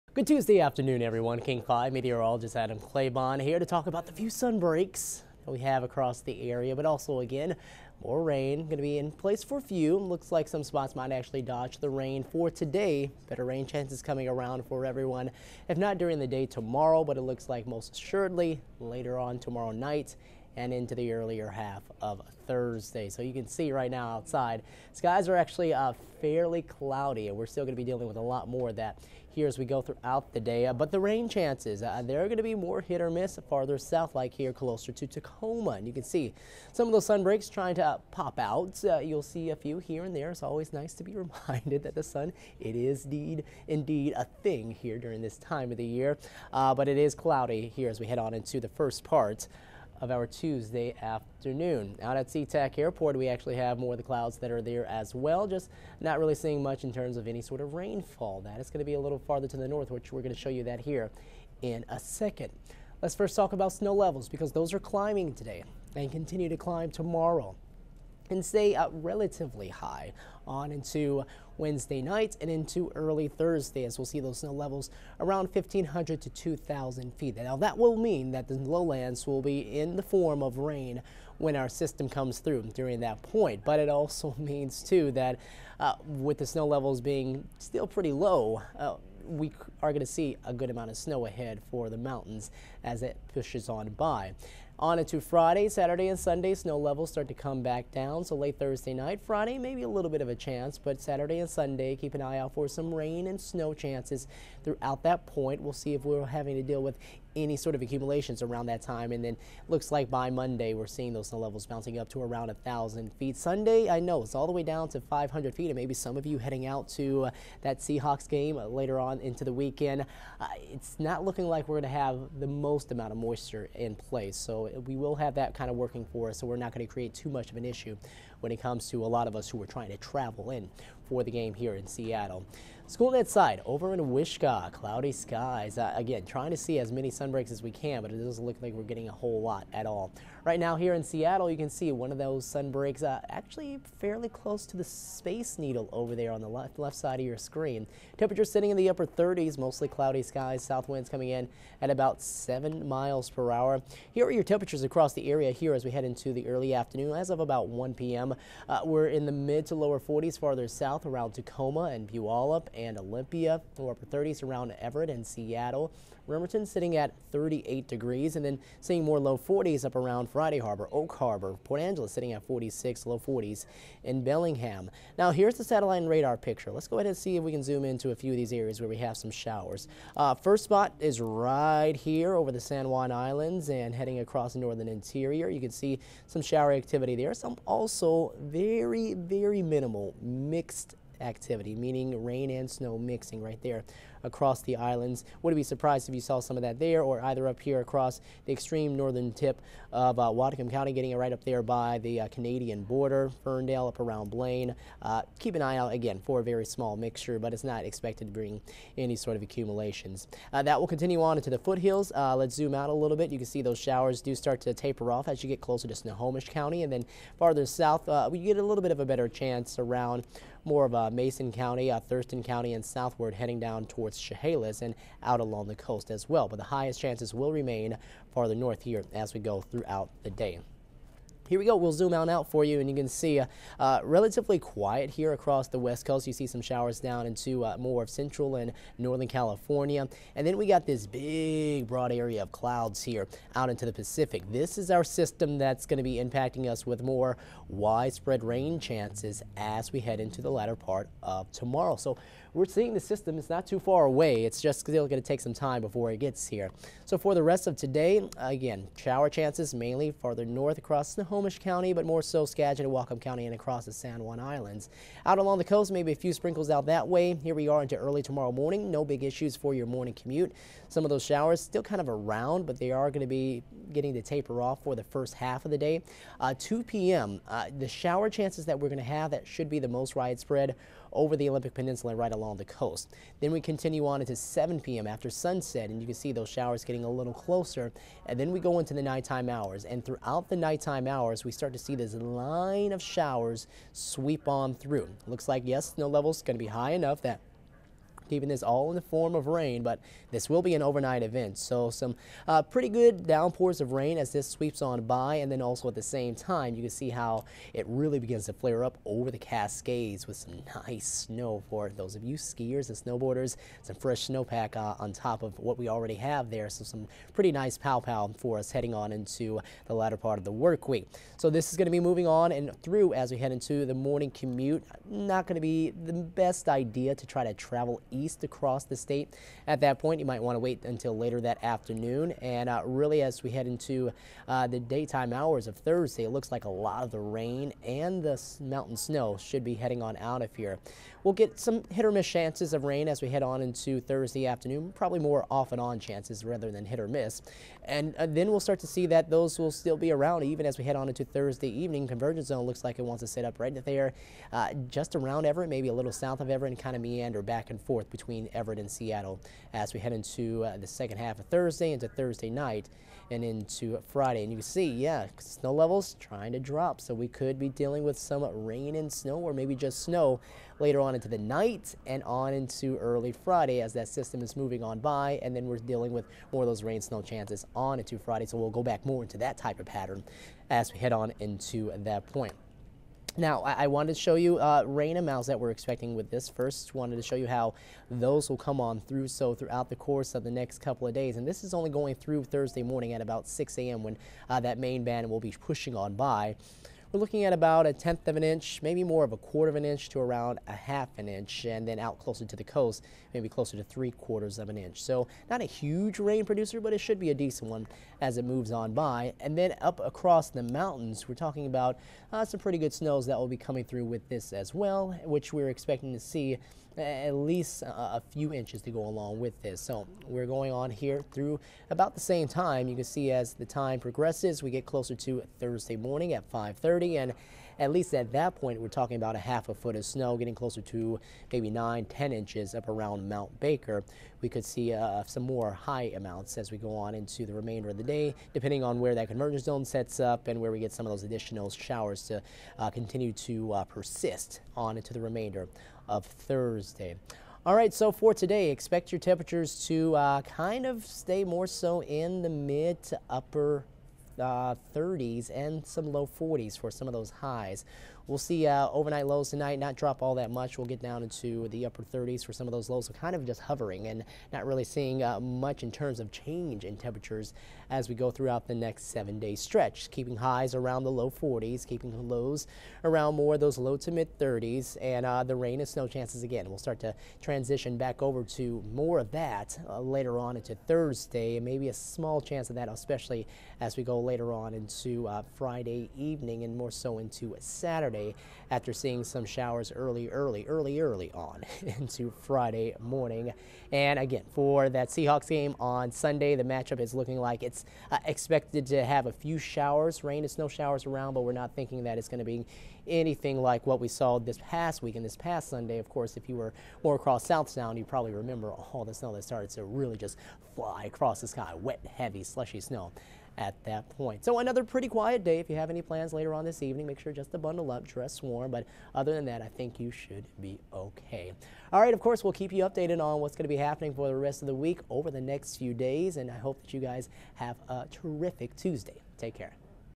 Spotty showers and rising snow levels | KING 5 weather
12/6/22 noon forecast with Meteorologist Adam Claibon.